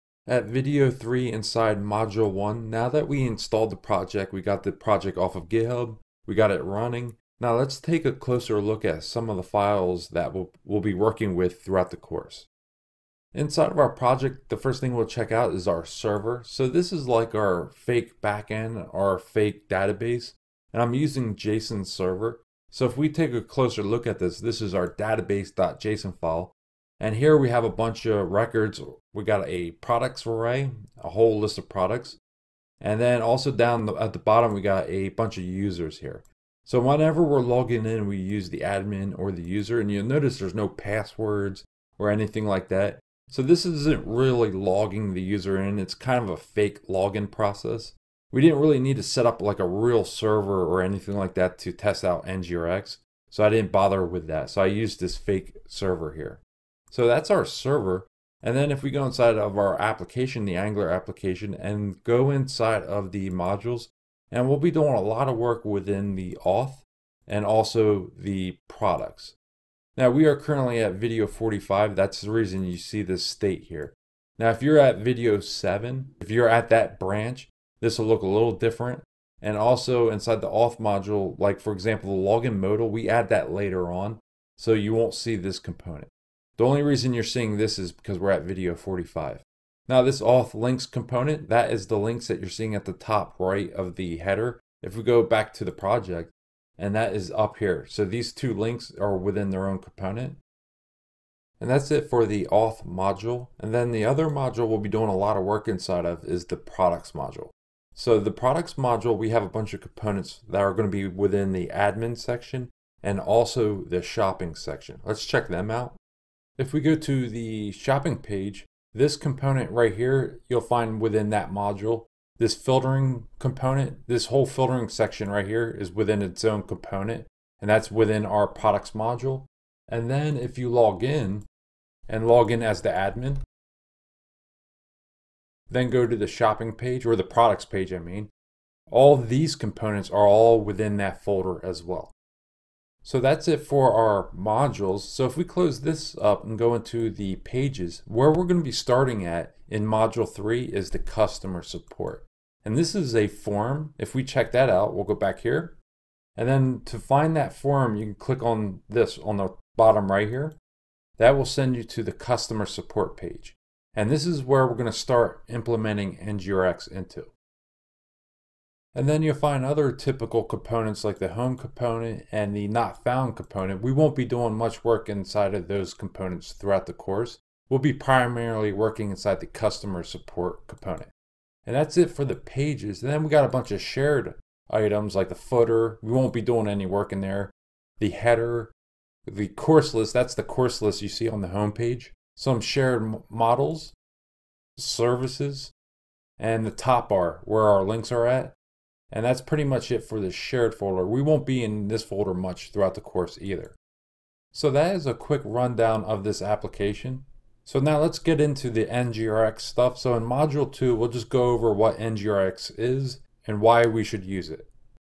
Video 3 Review NGRX Project
In this video, We review the project we will be working on throughout the course.